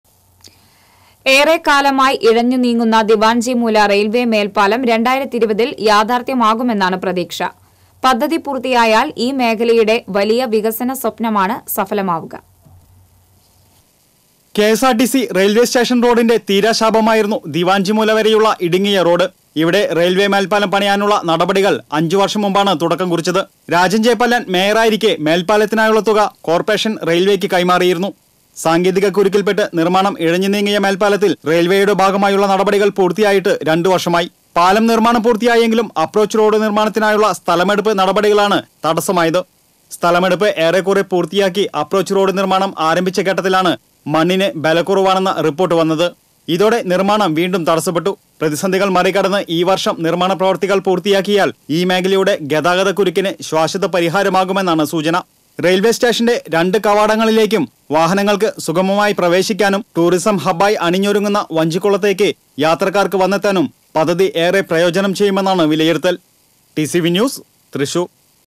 ഇഴഞ്ഞു നീങ്ങുന്ന ദിവാന്‍ജിമൂല റെയില്‍വേ മേല്‍പ്പാലം | TCV Thrissur
ഏറെക്കാലമായി ഇഴഞ്ഞു നീങ്ങുന്ന ദിവാന്‍ജിമൂല റെയില്‍വേ മേല്‍പ്പാലം 2020ല്‍ യാഥാര്‍ത്ഥ്യമാകുമെന്നാണ് പ്രതീക്ഷ. പദ്ധതി പൂര്‍ത്തീയായാല്‍ ഈ മേഖലയുടെ വലിയ വികസന സ്വപ്നമാണ് സഫലമാകുക